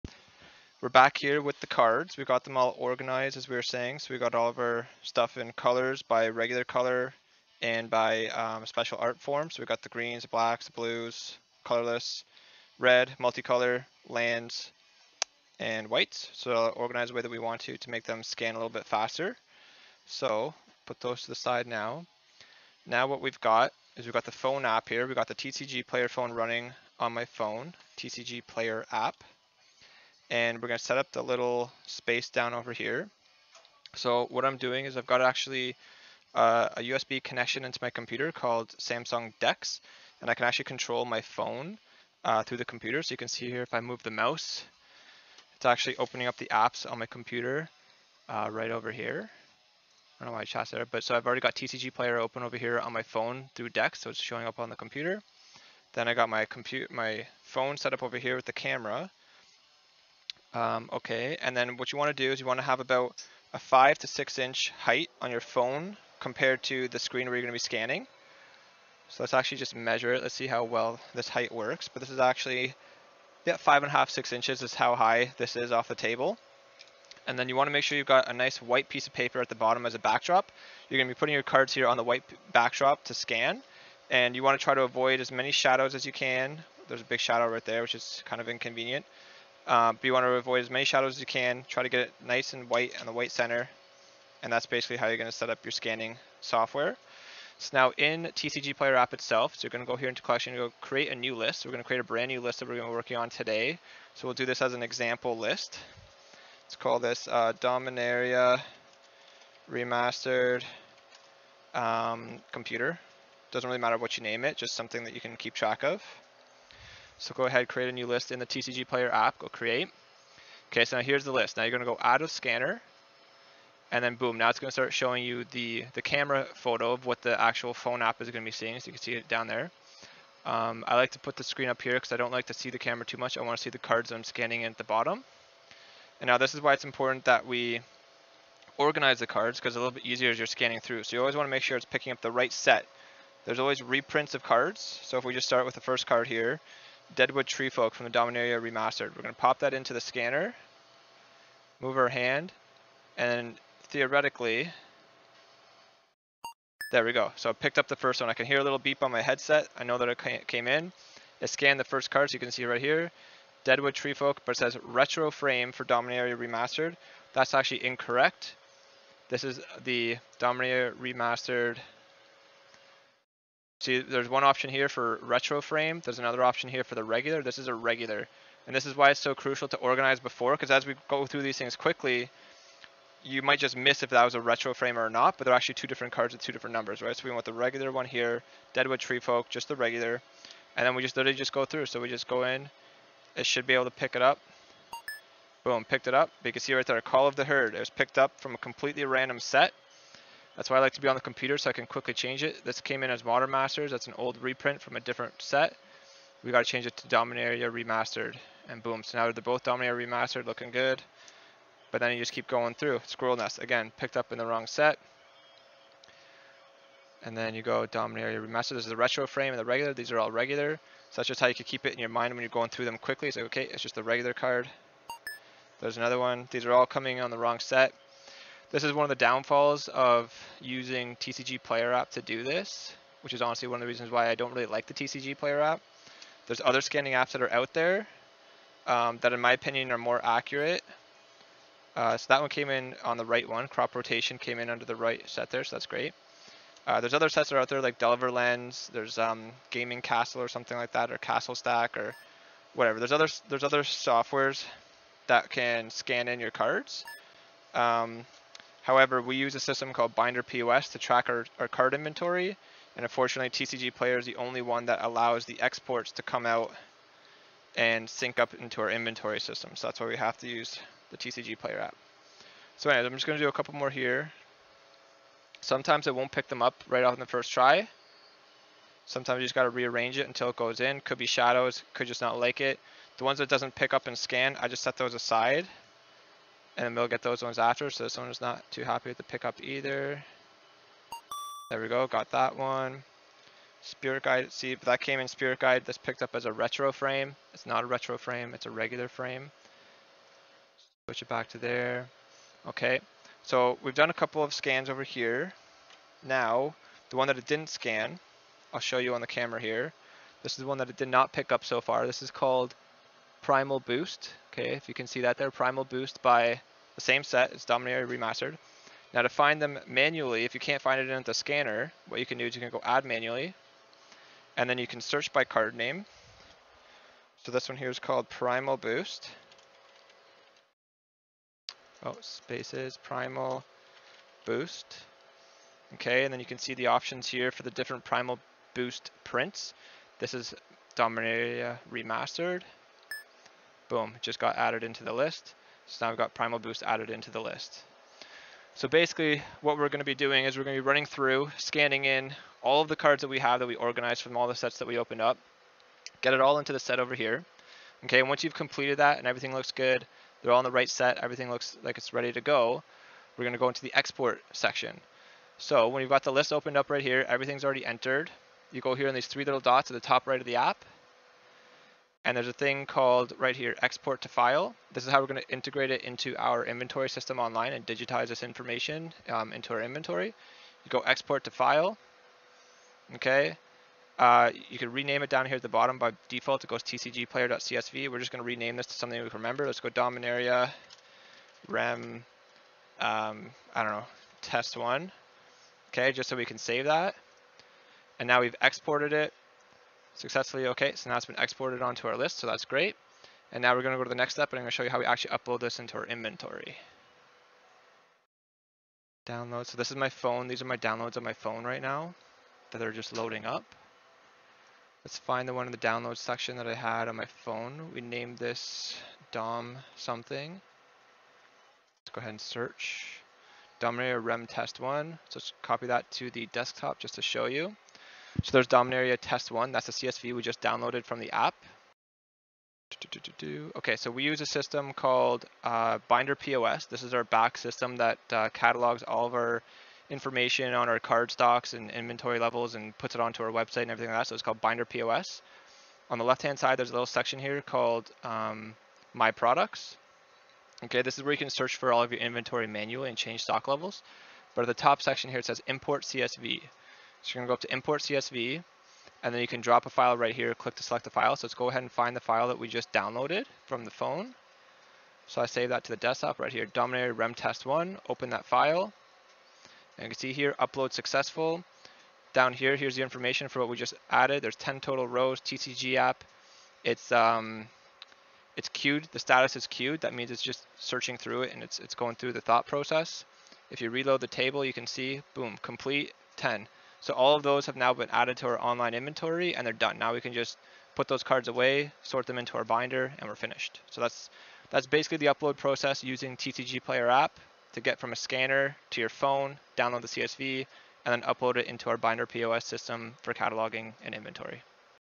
How to Scan and Catalog Your Cards - TCGplayer App (Magic, Pokemon & Yu-Gi-Oh Cards)
We explain the process in how we organize a batch of cards for scanning, how to scan them into TCGplayer App, then download the CSV to export/import into our inventory cataloging system. This is also known as digitizing your inventory. You can do this all with just your phone, or you can optionally connect your phone to a desktop computer for easier scanning.

Apps used in this video:
- TCGplayer App (to scan images of cards on your phone)
- Samsung Dex (to control your phone on a desktop computer: OPTIONAL)
- BinderPOS (our software to catalog and sell cards on our website & Ebay)

00:00 Organize your cards for faster scanning
00:23 Set up your scanning area
01:37 Start scanning a new card list on TCGplayer app
05:54 How to export your card list as a .CSV
07:12 How to import a .csv into BinderPOS Inventory

This method is useful for game stores looking to quickly scan in their card collections to resell. Its also useful for individual collectors/hobbyists to catalog their collections and know its value.

Have questions or other methods you think are faster?